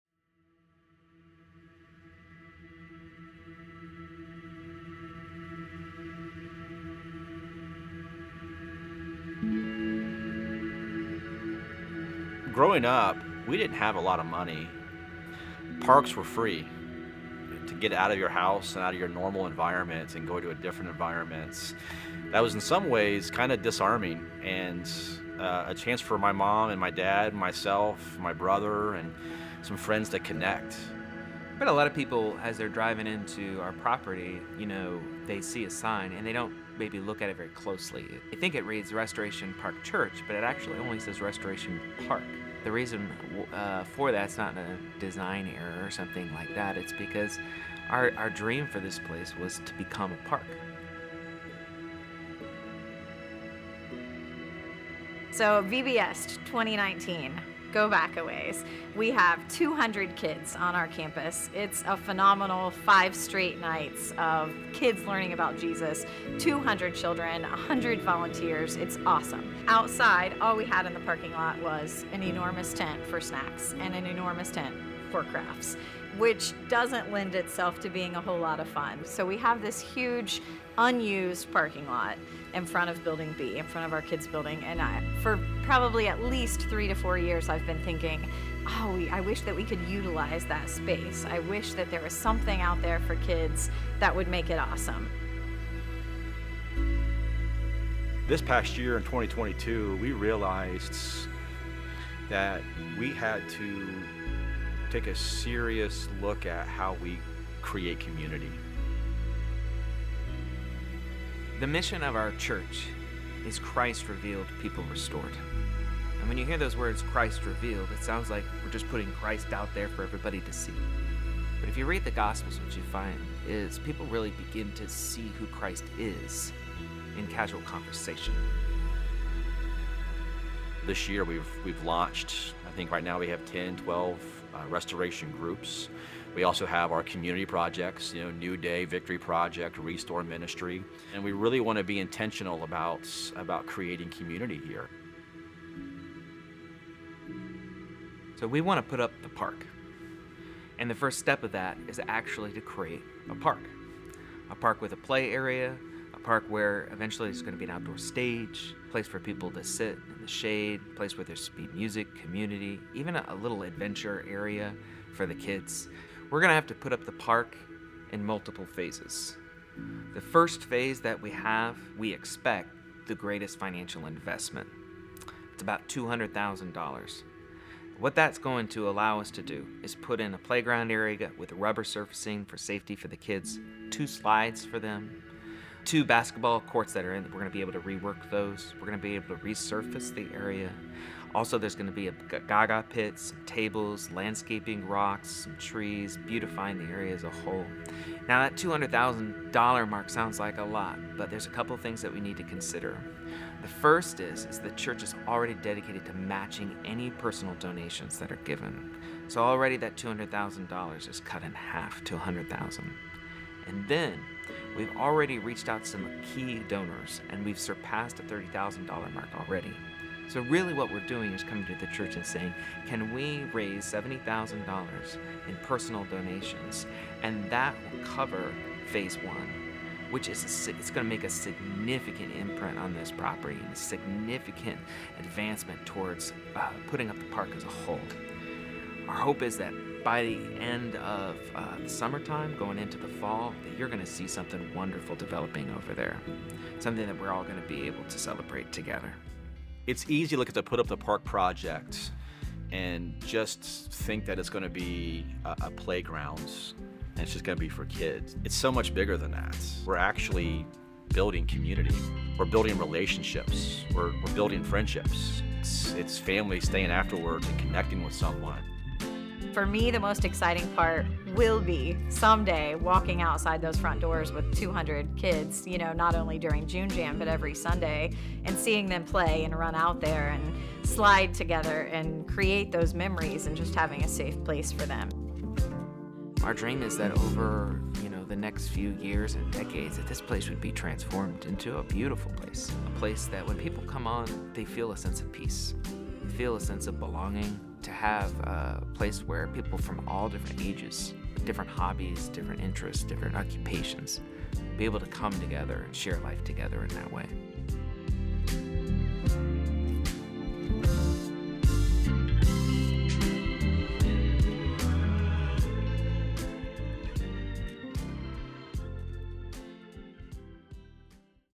Put up the Park
Come see what new things are happening at Restoration Park and how you can help.